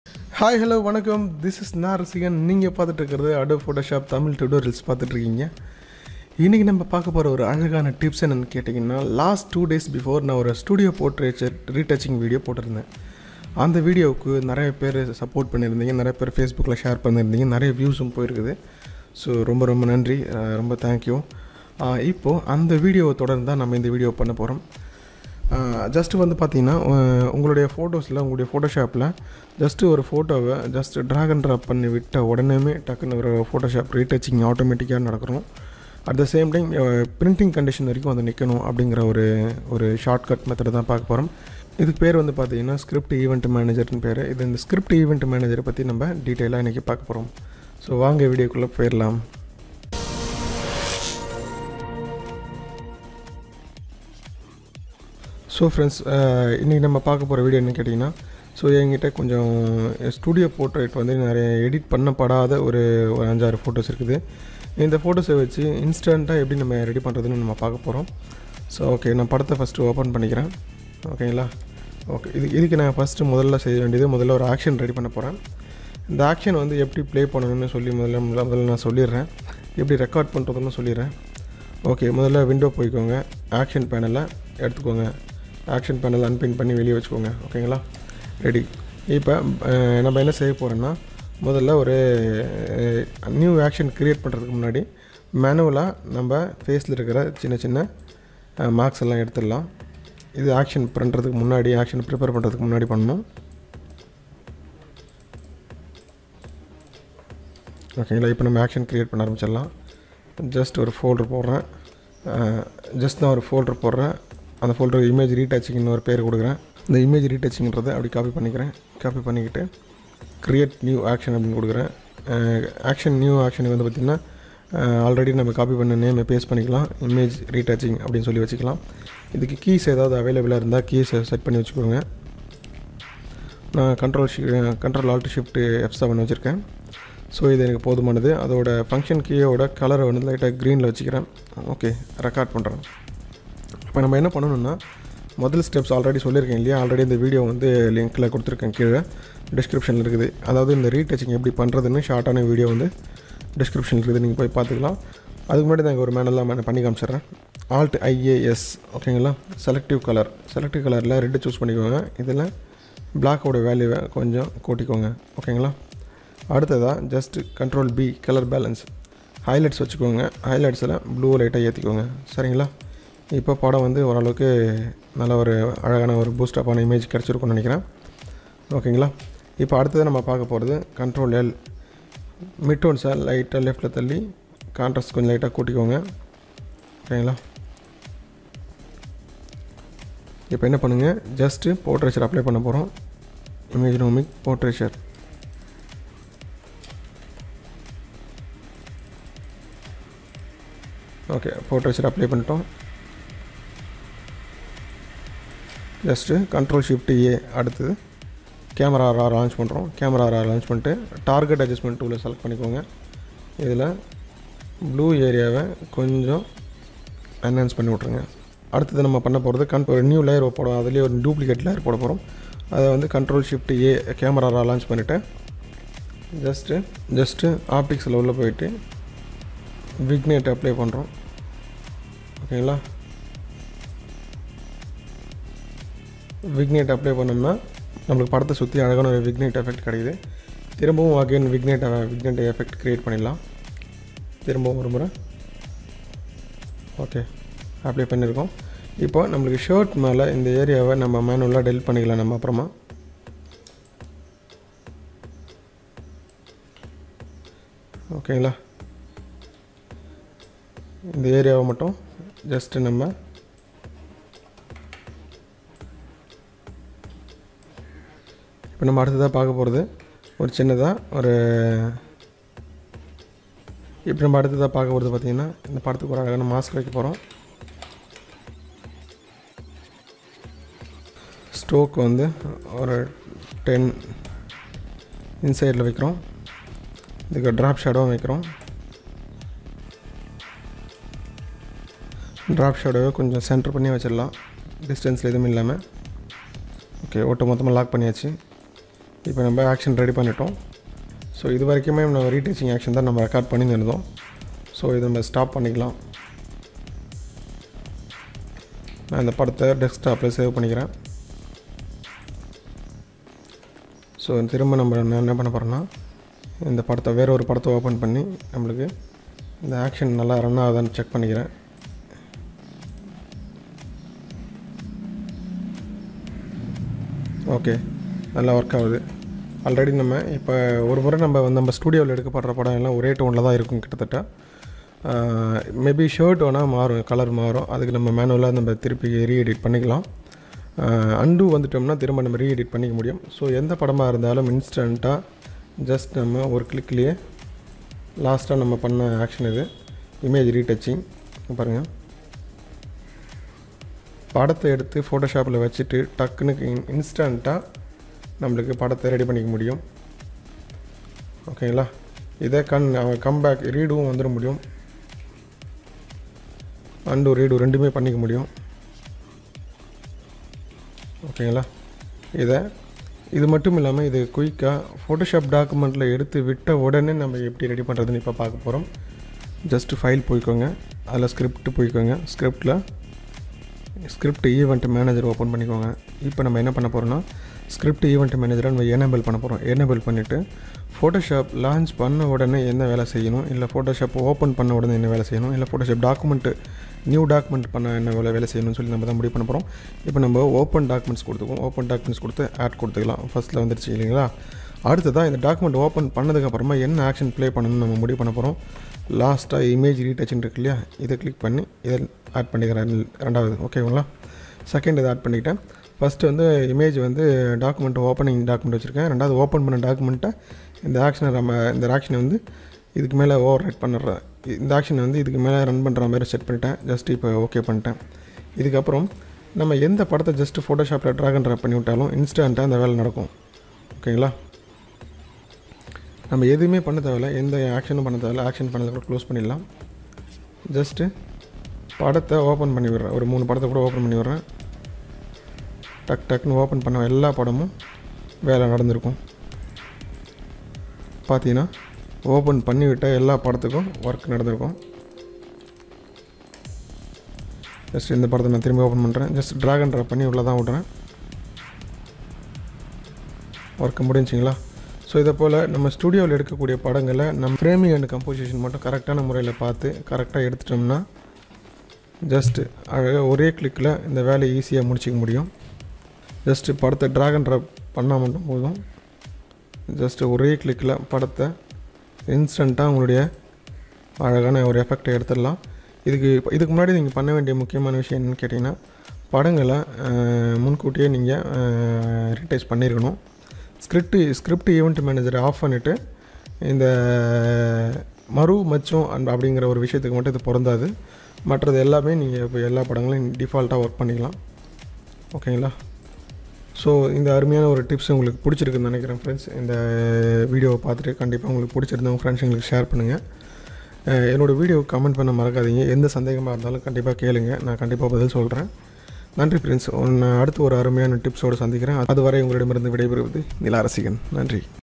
Photoshop Scripting Tutorial - ஆட்டோமேட்டிக்காக 5 வினாடியில் Portrait Retouching செய்வது எப்படி…..
நமது ஸ்டுடியோக்களில் எடுக்கப்படும் படங்களை நேரம் விரயம் செய்யாமல் விரைவாக 5 வினாடியில் எப்படி Portrait Retouching செய்வது என்பது குறித்து விளக்கமாக கூறியுள்ளோம்...காணத்தவறாதீர்…..

 தங்களின் மேலான கருத்துக்களை கமெண்டில் வெளியிடுங்கள்…...